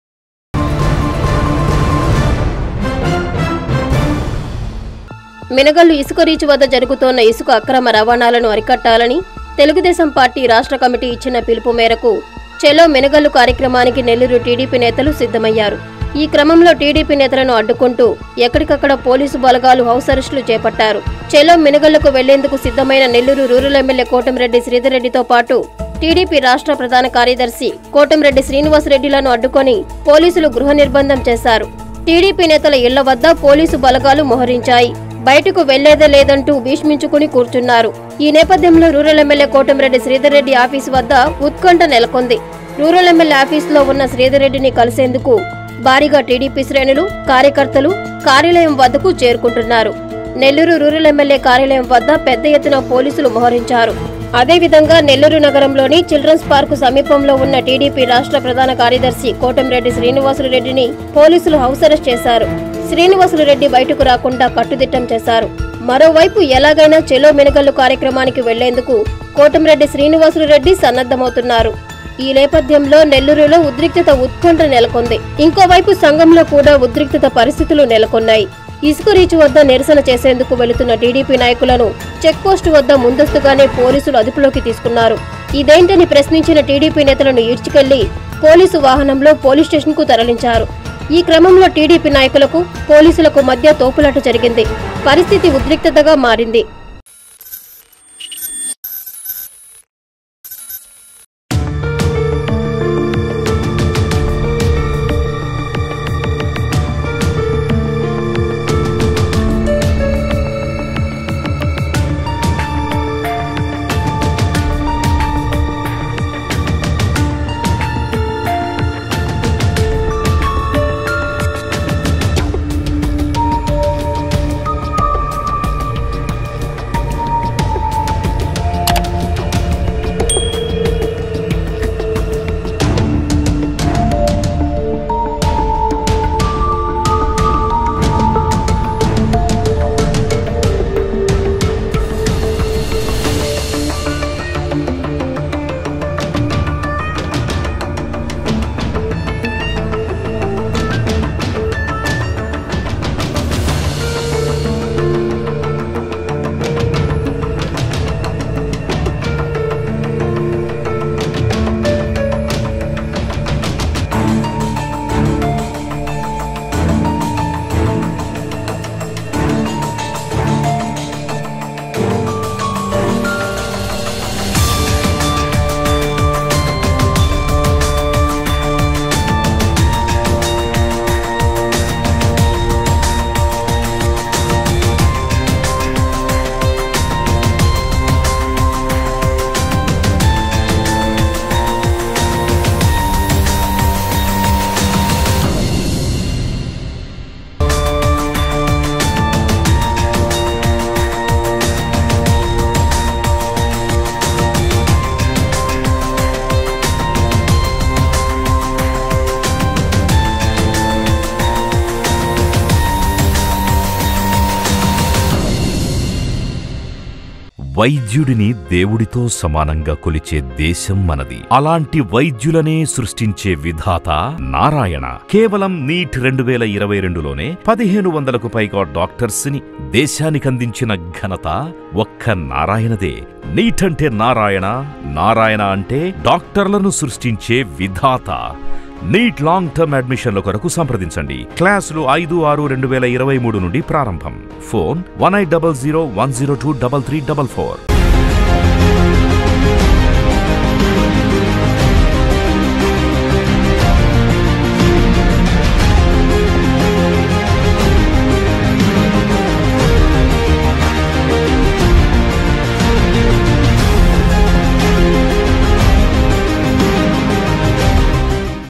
నెల్లూరులో టీడీపీ నేత‌లు హౌస్అరెస్ట్‌ | Nellore Rural MLA House Arrest | TDP vs YCP
- రూర‌ల్ ఎమ్మెల్యే కోటంరెడ్డి శ్రీ‌ధ‌ర్‌రెడ్డి ఆఫీస్ వ‌ద్ద ఉత్కంఠ‌
- టీడీపీ రాష్ట్ర ప్రధాన కార్యదర్శి కోటంరెడ్డి శ్రీనివాసులురెడ్డి గృహ‌నిర్భందం
- టీడీపీ నేత‌ల ఇళ్ల వ‌ద్ద మొహ‌రించిన పోలీసు బ‌ల‌గాలు
- ఛ‌లో మిన‌గ‌ల్లు కార్య‌క్ర‌మాన్ని అడ్డుకునేందుకు య‌త్నం
- ఎలాగైనా ఛ‌లో మిన‌గ‌ల్లు వెళ్లేందుకు స‌న్న‌ద్ద‌మైన టీడీపీ నేత‌లు